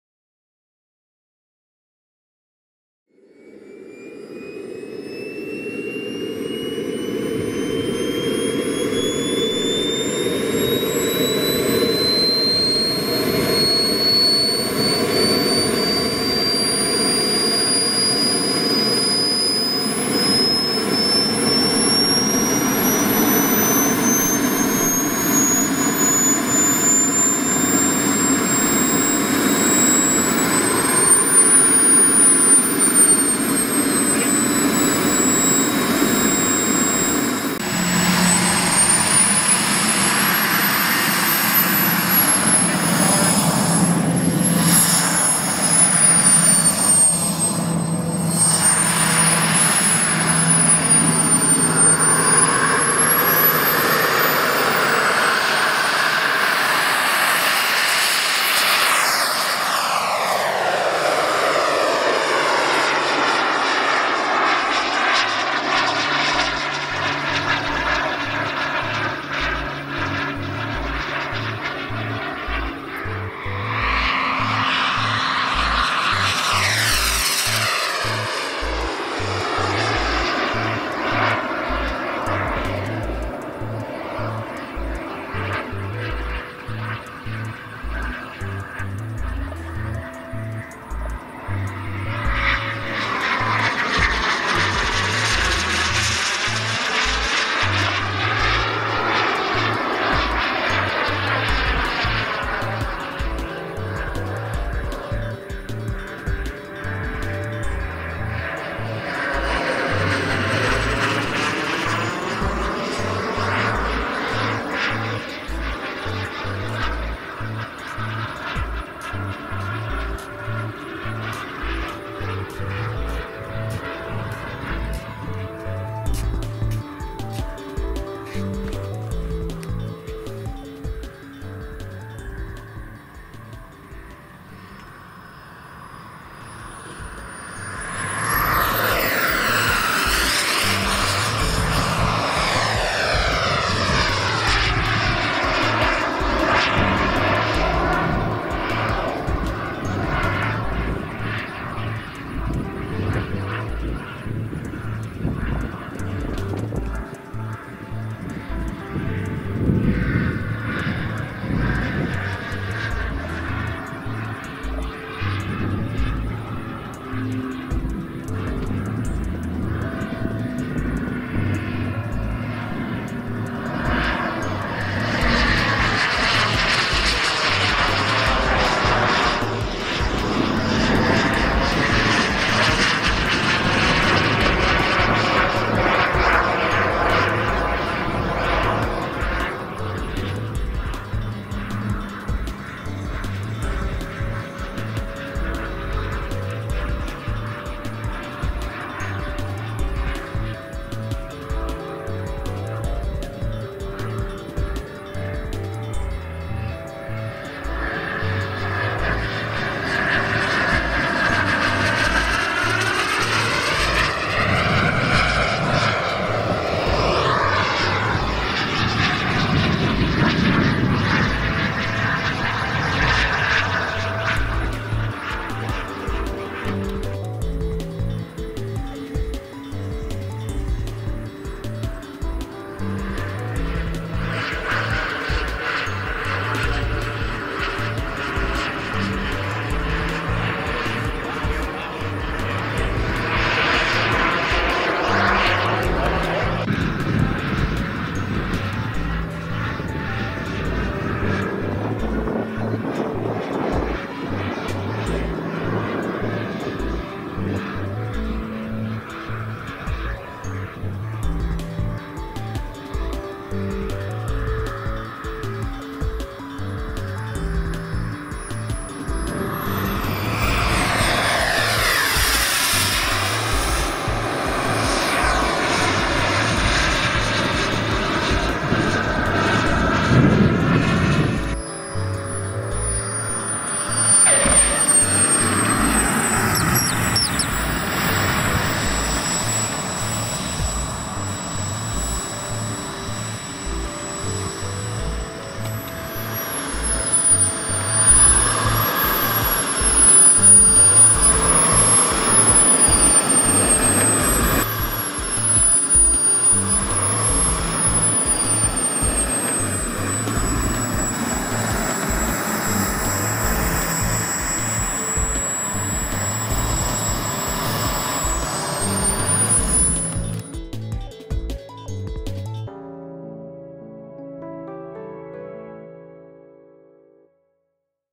Delta Wing RC Jet 210 size
It's my 1st time seeing a 210 size Jet up close and in action. What an experience it was. I did not get any of the pilots info other than the fact that the turbine was a 210 size. It has fully functional canards and thrust vectoring. Next time I'll surely demand he shows us some flat spins and high alphas! If anyone knows him, feel free to invite him to the channel and enter some of the specs.